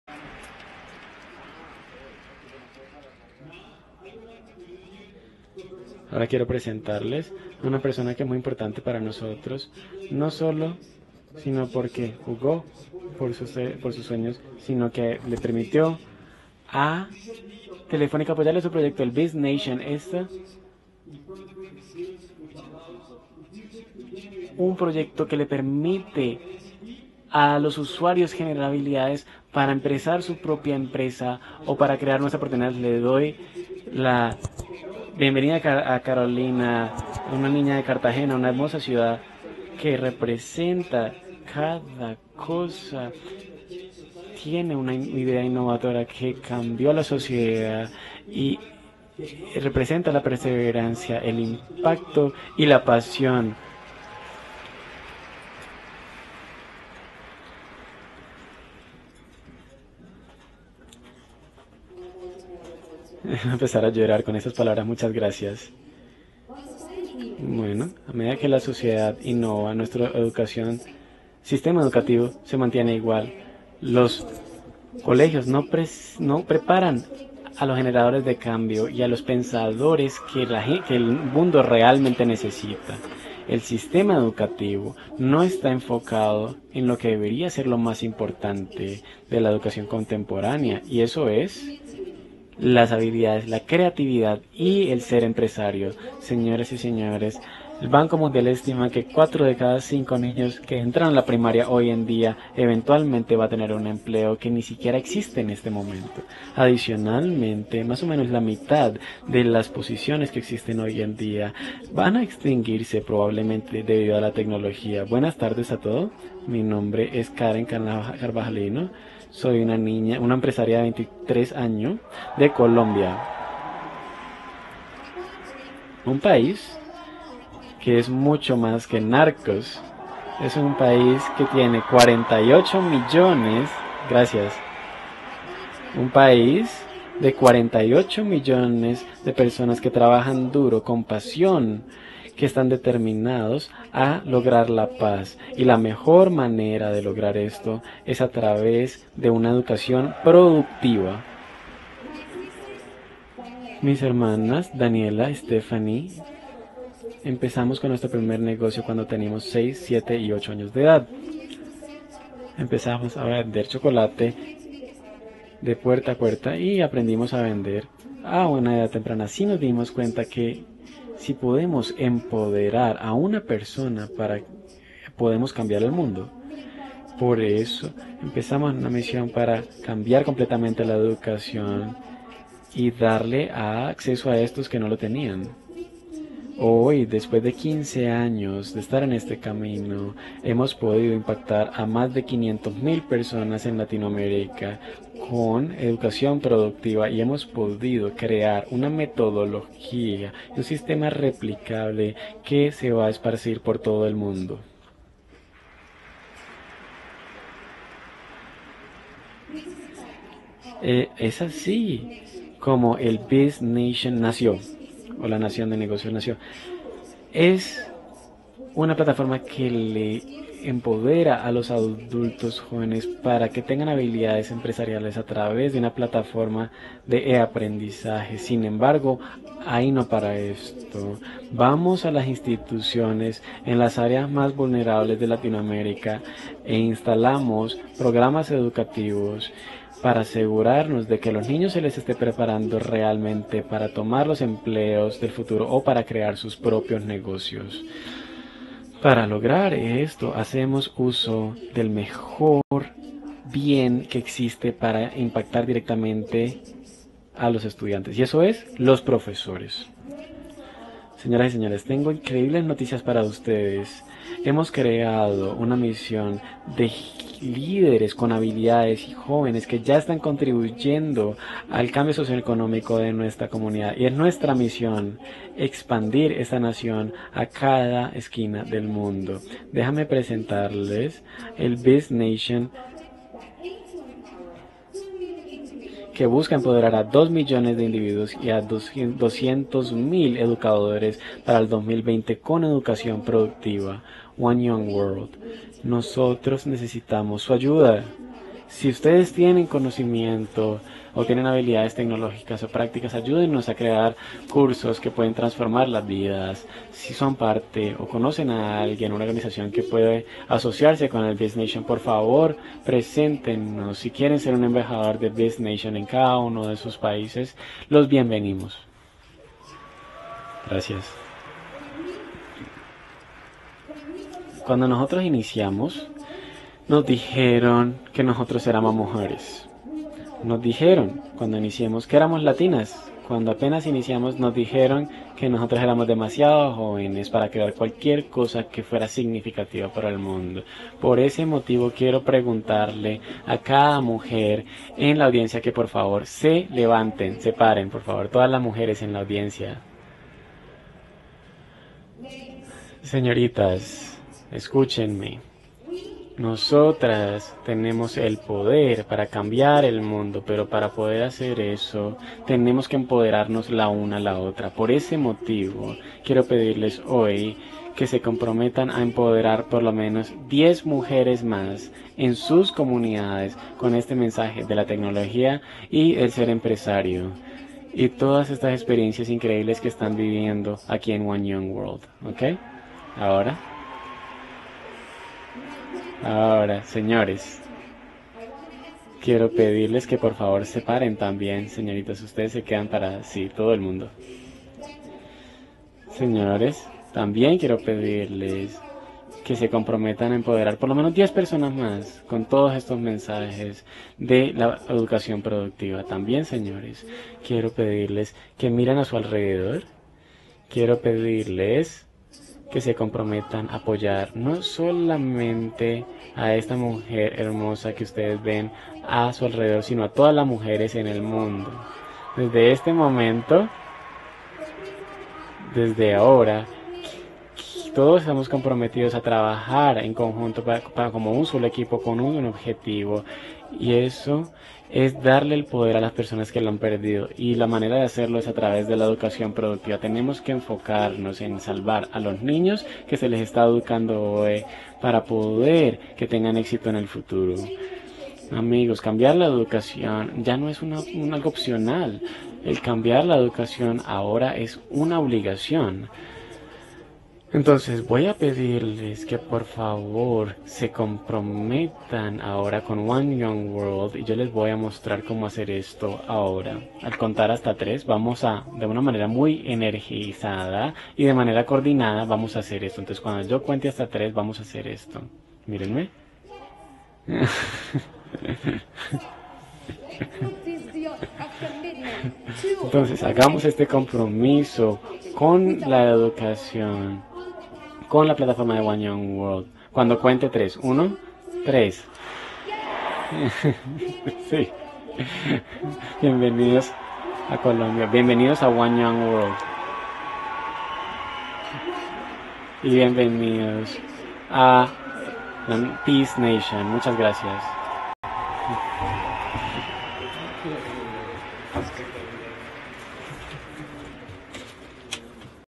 Karen Carvajalino representante de @thebiznation - Día 3
Alfonso Gómez, Presidente CEO de Telefónica Movistar presentó en One Young World a Karen Carvajalino de @thebiznation_ un emprendimiento que cuenta con el apoyo de Open Future de Telefónica.